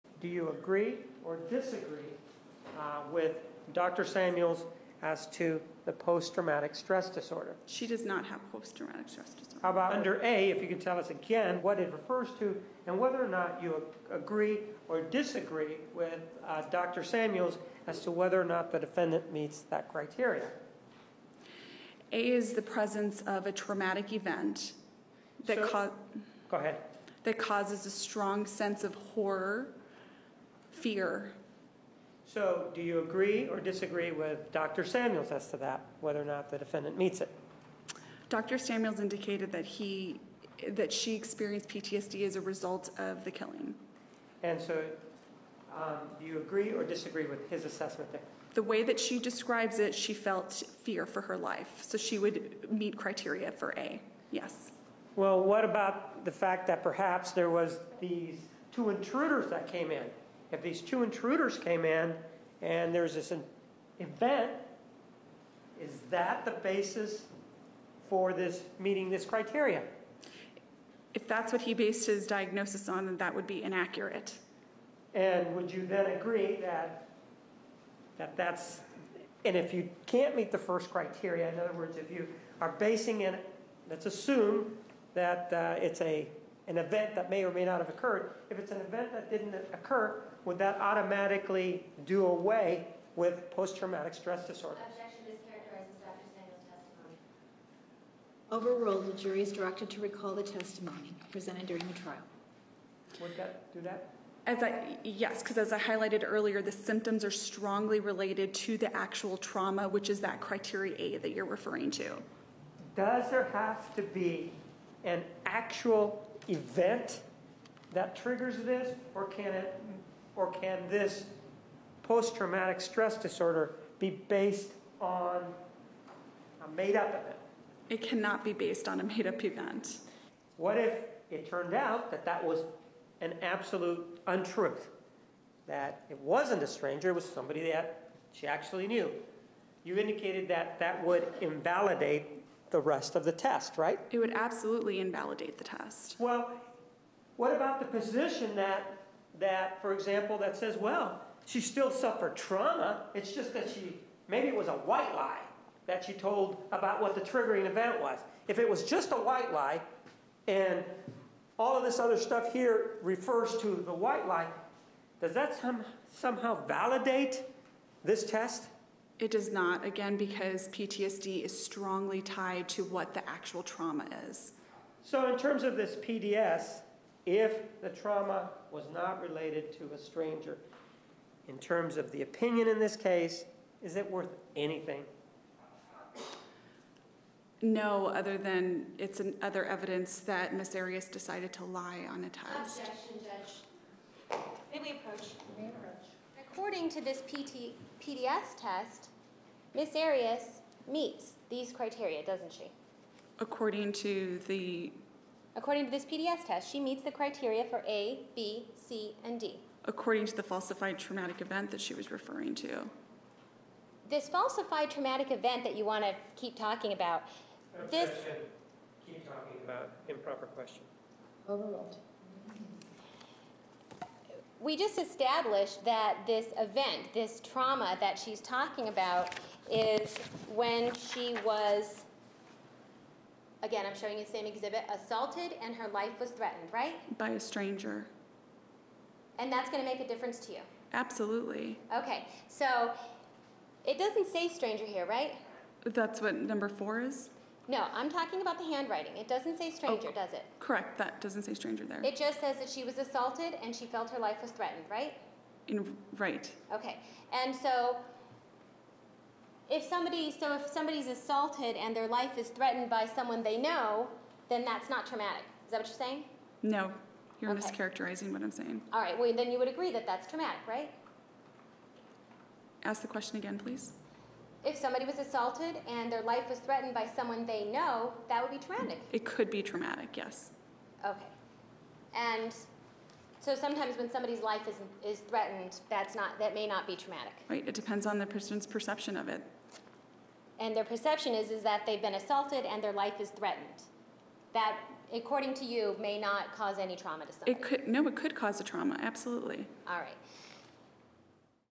Janeen DeMarte Disagrees That Jodi Arias Had PTSD -- Defense Says She Lied But Still Suffered Trauma
The prosecution's first rebuttal witness, psychologist Janeen DeMarte, disagrees with the defense's psychologist witness Richard Samuels on his diagnosis that Jodi Arias suffered PTSD (post-traumatic stress disorder) resulting in memory loss after killing Travis Alexander.  Demarte's argument is that if Jodi lied during the test (saying that 2 intruders killed Travis), then it automatically voids the test because it cannot be based on something that is made up.  Jodi's defense lawyer, Jennifer Willmott, argues that even if Jodi lied about intruders, she would still have suffered trauma since she had just killed someone close to her (and was allegedly in fear for her life before doing so).  An argument can be made that Jodi planned to kill Travis, murdered him, and after that it doesn't matter whether she suffered PTSD or not.

Arizona vs Jodi Arias
Dr. Janeen DeMarte